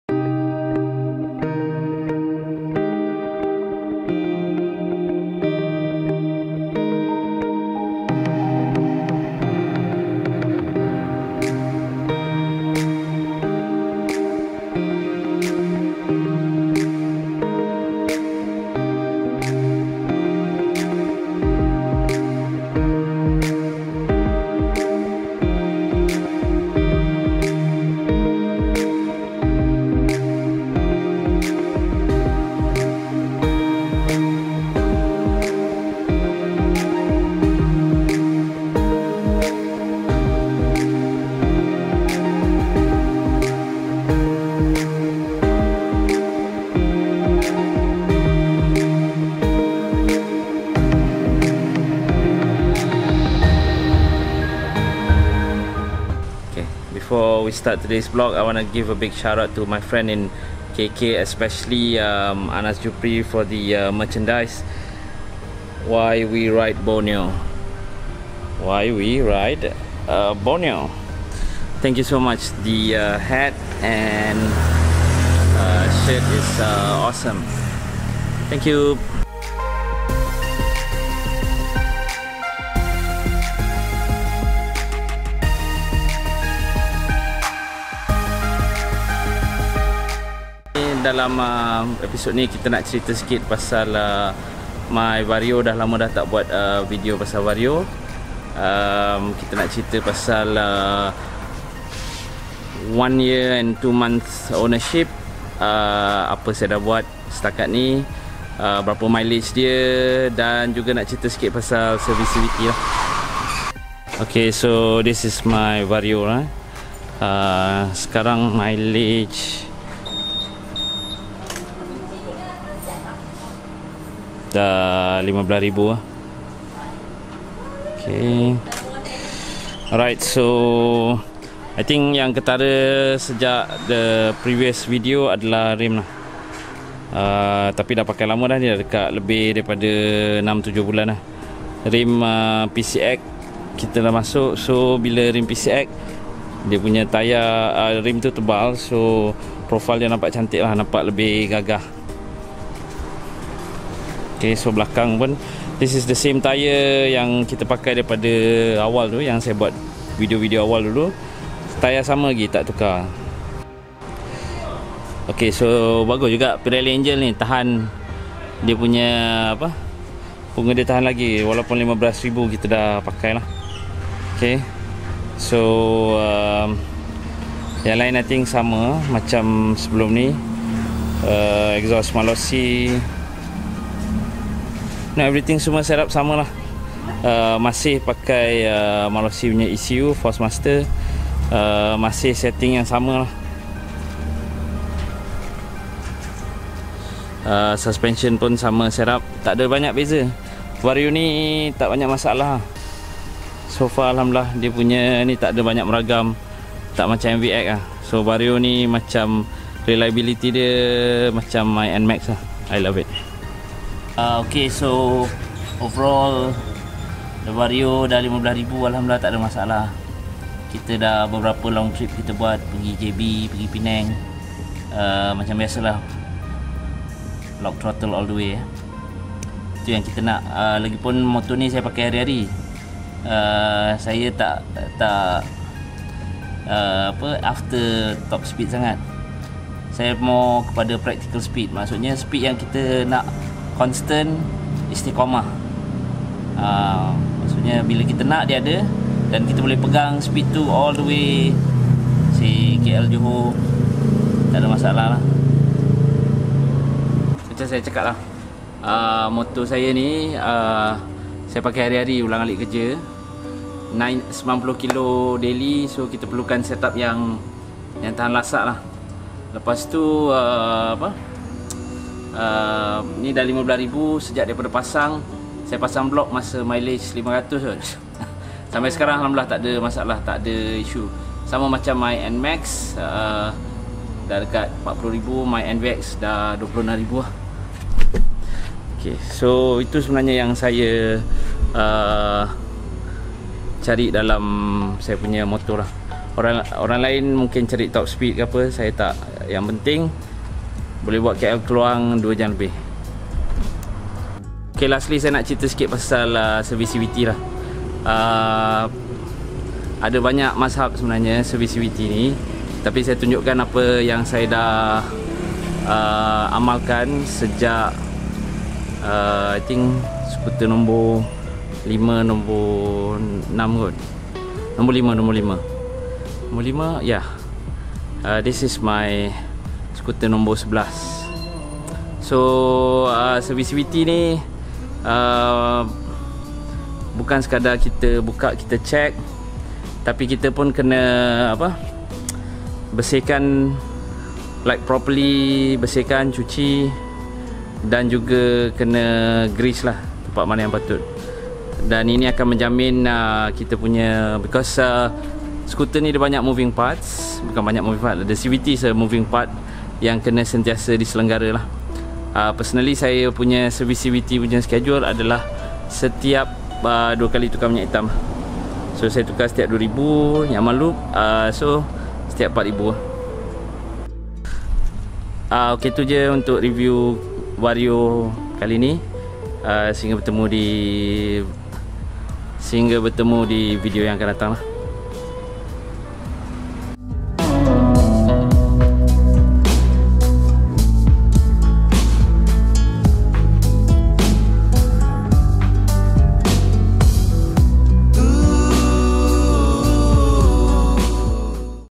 Honda Vario / Click - CVT Service & 1 year review
Hai semua. Episod ni kita belajar pasal servis CVT ye.